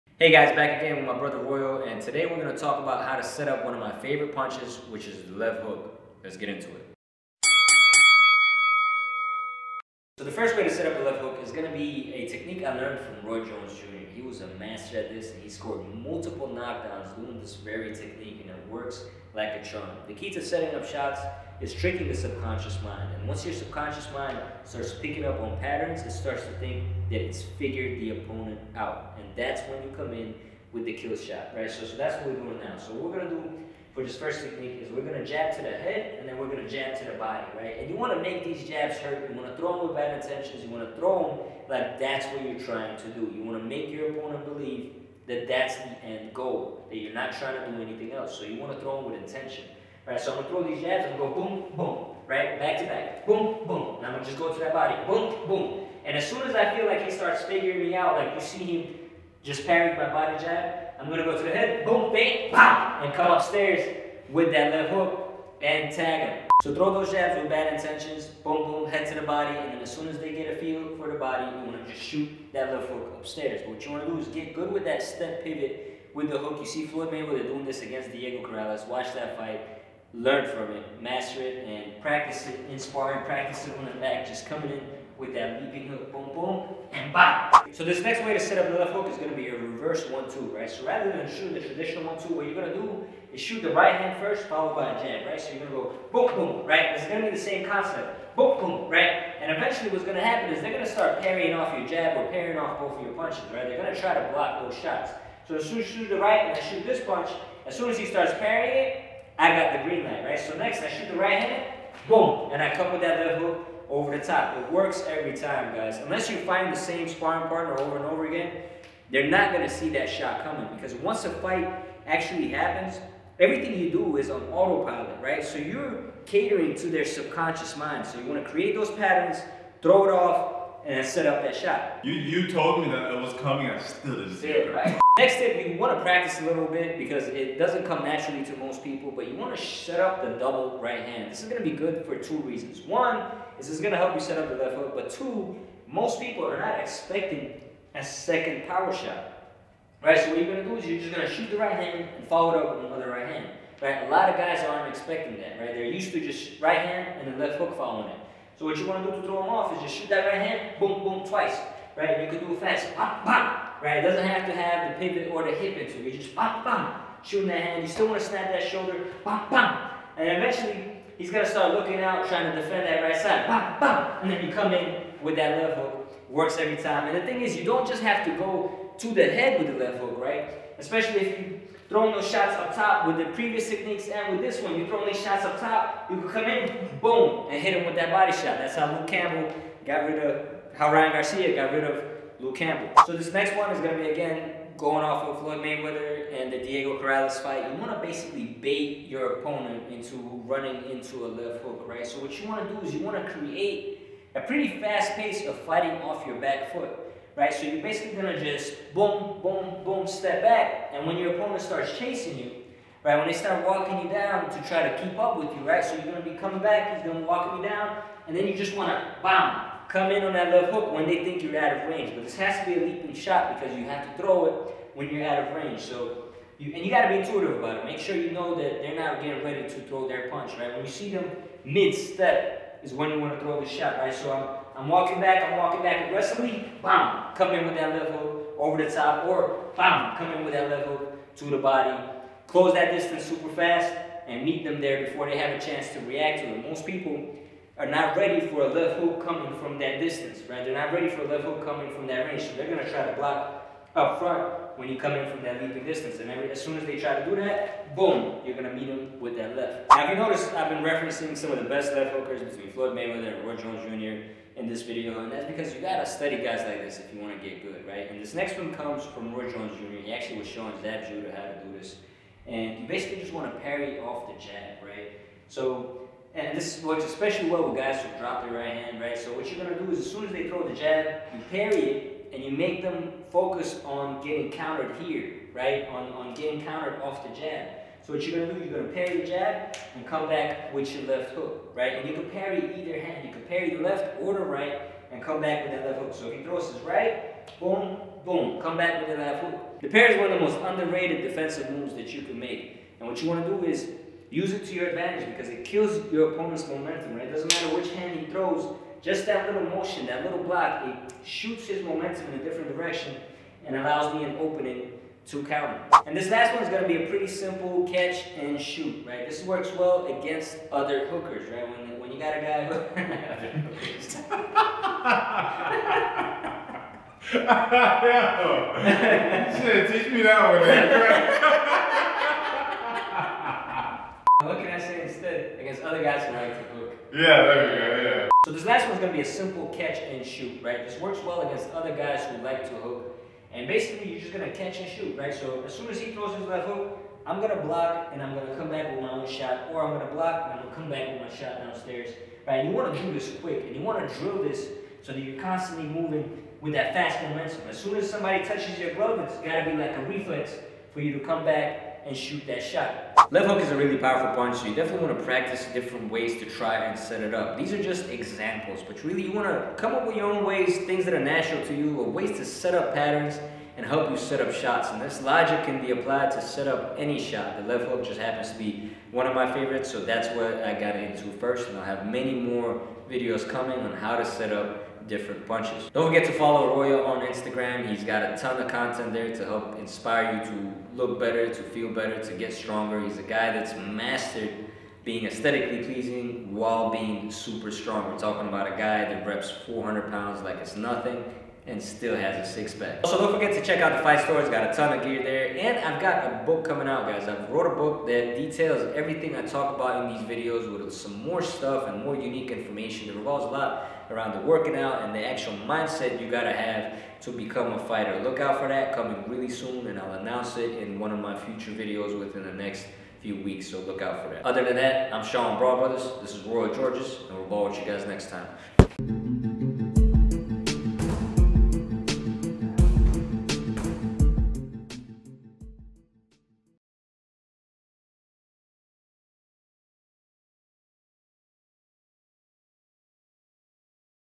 How to Set up the Left HOOK!
In this video I talk about how to set up the left hook.

What you want to do is use the pattern technique to help trick your opponents mind into thinking you're going to throw something else, and throw them off guard.

In this video we take tips from Floyd Mayweather, Roy Jones Jr, and other fighters.